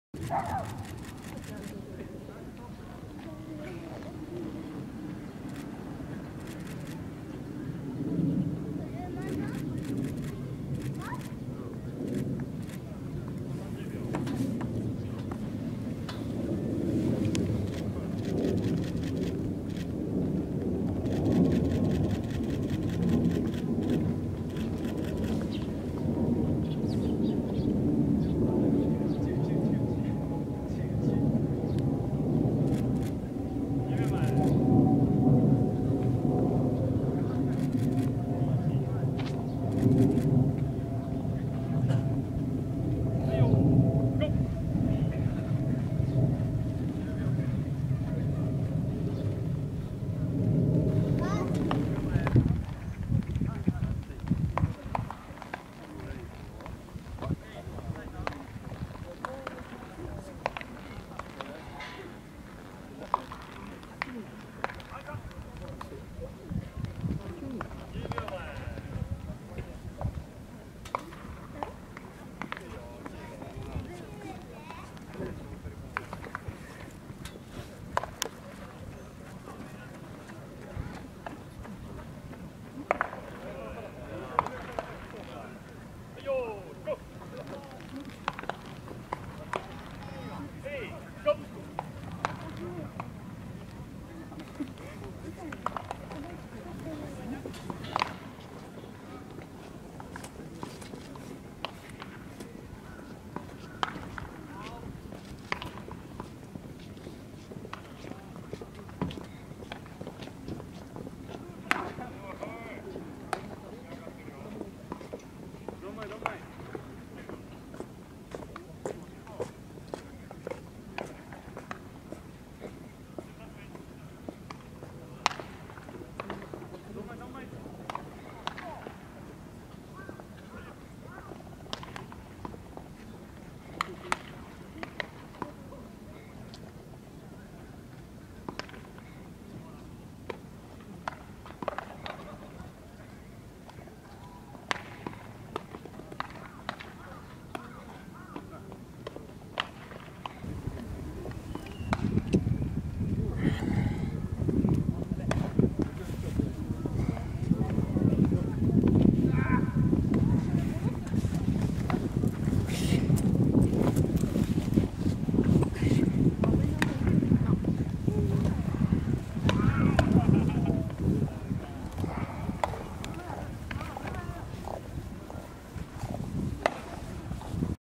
日ハム 堀瑞輝・高山優希選手など新人合同自主トレ（鎌ヶ谷スタジアム）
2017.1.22 鎌ヶ谷スタジアム
ドラフト1位の堀瑞輝選手をはじめ、ルーキー達が元気よく練習をしていました。堀選手はまだあどけなさが残ってプロという感じはまだありませんね（まだ高校生ですから当たり前かな）。
その点、大谷翔平選手なんかは、やはりオーラさえ（笑）
このルーキー達が早く1軍で活躍する姿をみたいものです。